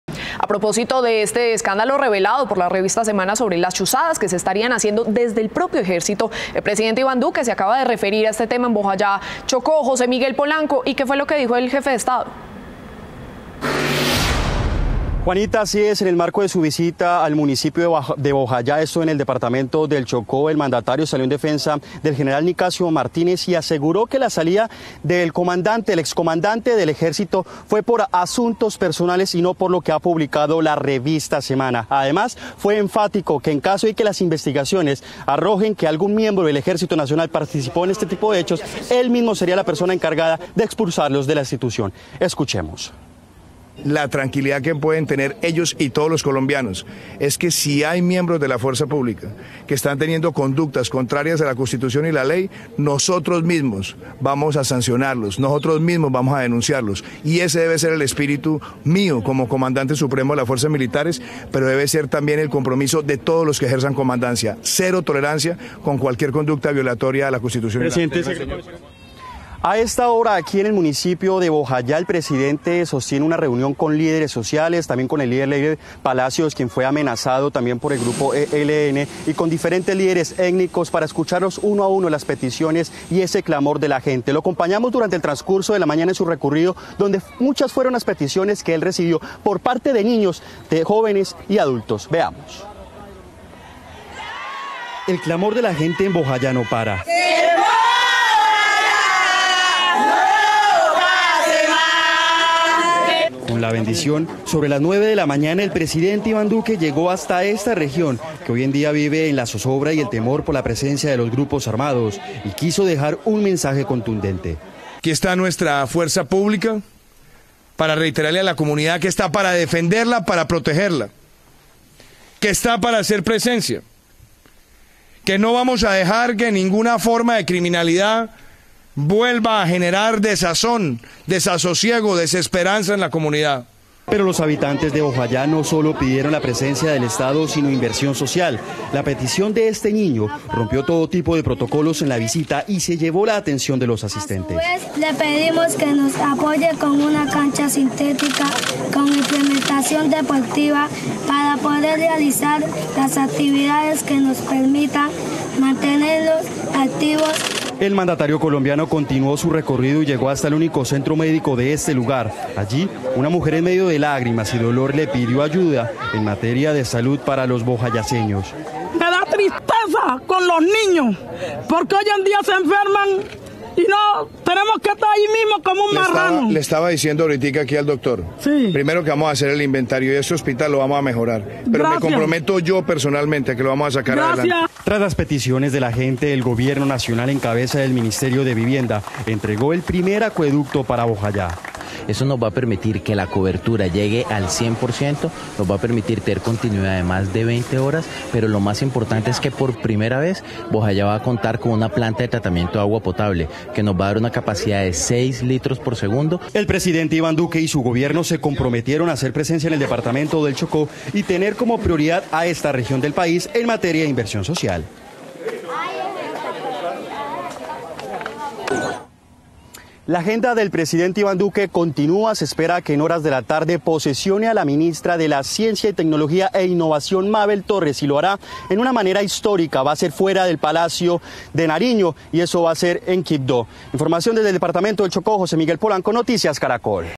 Llorando, mujer en Bojayá le pidió al presidente Iván Duque mejorar sistema de salud
El mandatario recorrió esta región del Chocó y se comprometió a tener más presencia del Estado.